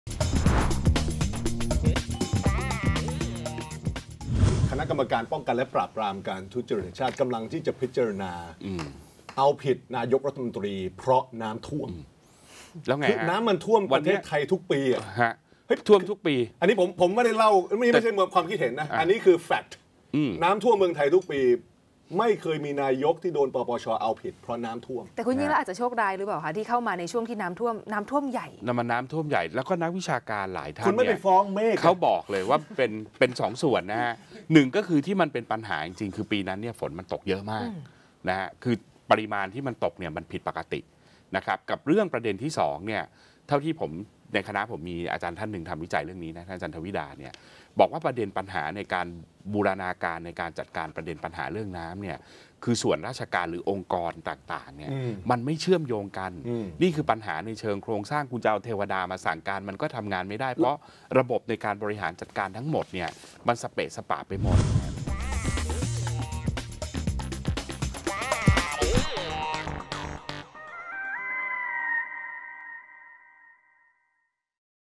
แนะนำ ป.ป.ช. ไปฟ้องเทวดา แก้น้ำท่วม !
คลิกชมรายการเต็มได้ที่

น้ำท่วมประเทศทุกปี แต่ ป.ป.ช. ดันหยิบเรื่องนี้มาเอาผิดอดีตนายกฯยิ่งลักษณ์ ถือเป็นประเด็นที่ไม่เคยมีนายกรัฐมนตรีไทยคนไหนเคยโดนมาก่อน แค่นึกภาพตามก็ 'งง' แล้ว สงสัยต้องแนะนำ ป.ป.ช. ให้ไปฟ้องเทวดา เผื่อจะแก้ปัญหาได้